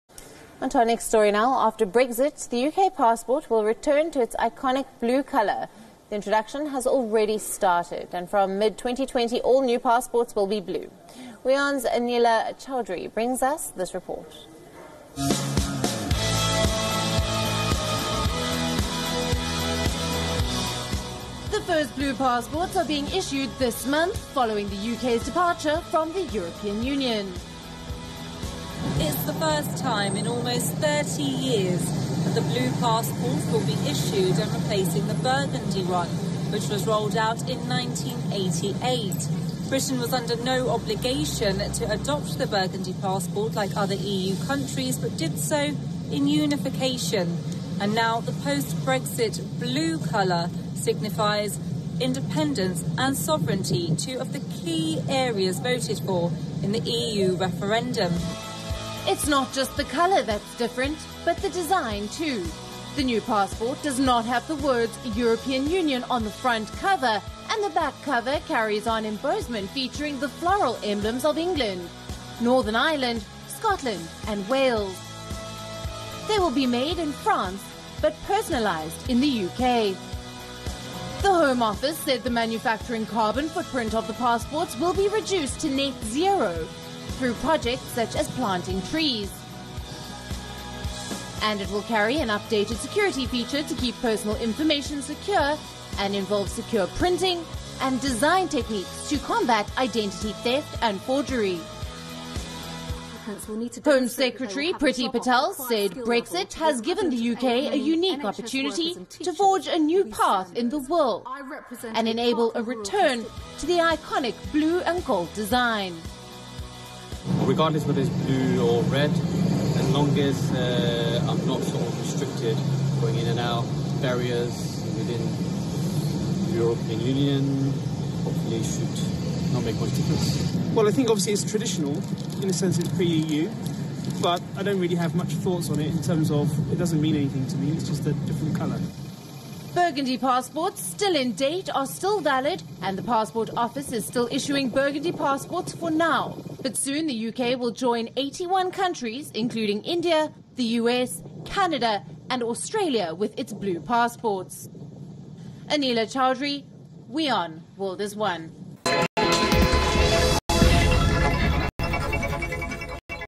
Your Story: UK passport switches from burgundy to blue
After Brexit, the United Kindom passport will return to its iconic blue colour. Watch WION's report on the story.

WION: shorturl.at/fwKO0
Zee News English: shorturl.at/aJVY3
Zee News Hindi: shorturl.at/eorM1
Zee Business: shorturl.at/hpqX6
DNA News: shorturl.at/rBOY6
BGR: shorturl.at/eioqL